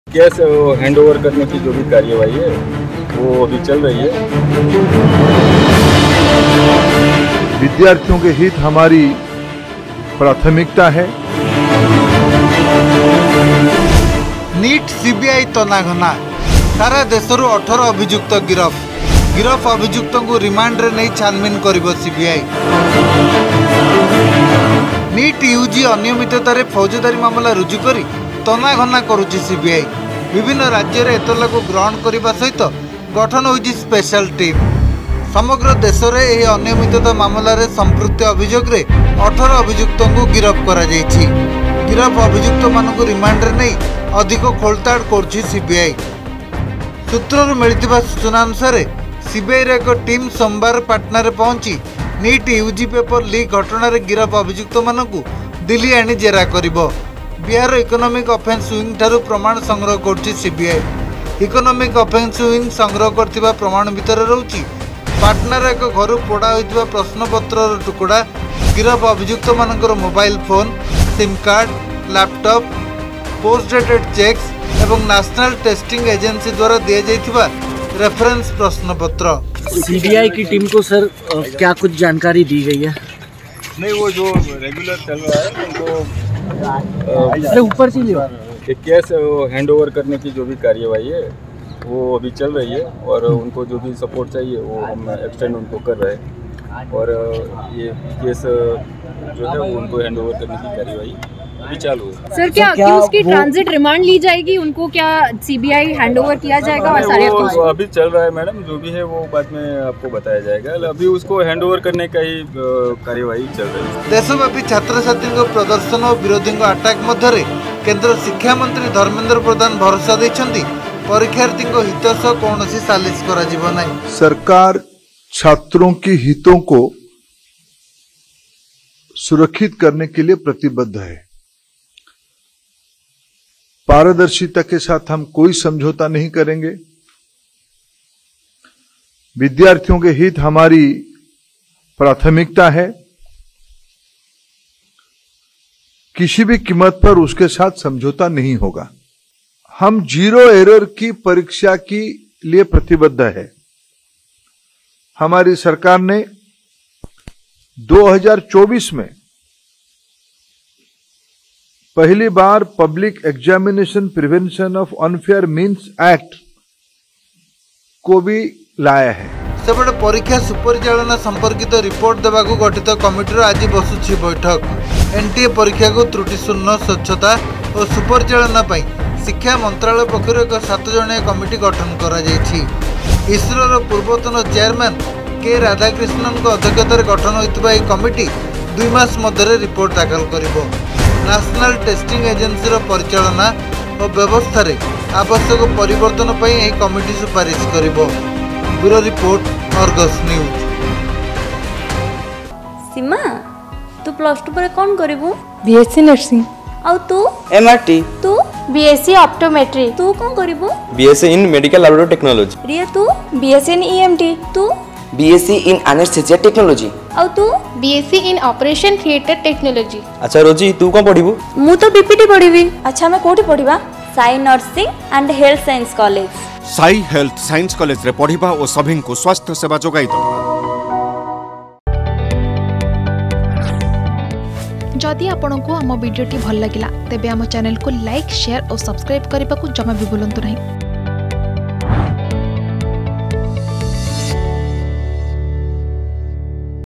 ନିଟ ୟୁଜି ଅନିୟମିତତାରେ ଫୌଜଦାରୀ ମାମଲା ରୁଜୁ କରି ତନାଘନା କରୁଛି ସିବିଆଇ ।
ସାରା ଦେଶରୁ 18 ଅଭିଯୁକ୍ତ ଗିରଫ ହୋଇଛନ୍ତି । ଗିରଫ ଅଭିଯୁକ୍ତଙ୍କୁ ରିମାଣ୍ଡରେ ନେଇ ଛାନଭିନ୍ କରିବ ସିବିଆଇ । କେନ୍ଦ୍ର ଶିକ୍ଷାମନ୍ତ୍ରୀ ଧର୍ମେନ୍ଦ୍ର ପ୍ରଧାନ ଭରସା ଦେଇଛନ୍ତି, ପରୀକ୍ଷାର୍ଥୀଙ୍କ ହିତ ସହ କୌଣସି ସାଲିସ କରାଯିବ ନାହିଁ ।